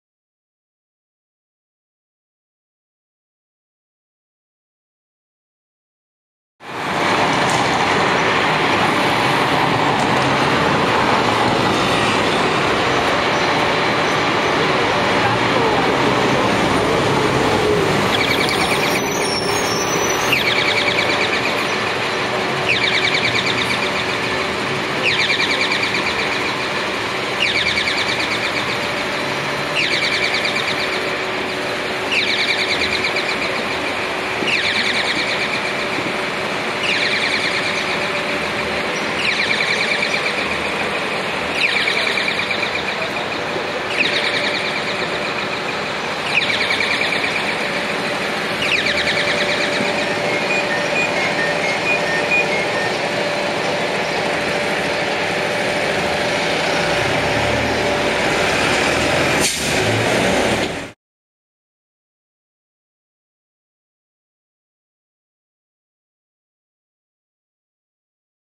Avenida Madero 9am 26 May 2015, Mexico City
A little film to show the world going to work in the Centro Historico, Cuidad de México. We had arrived at the airport at 5am, this my excuse for ragged frames  :-)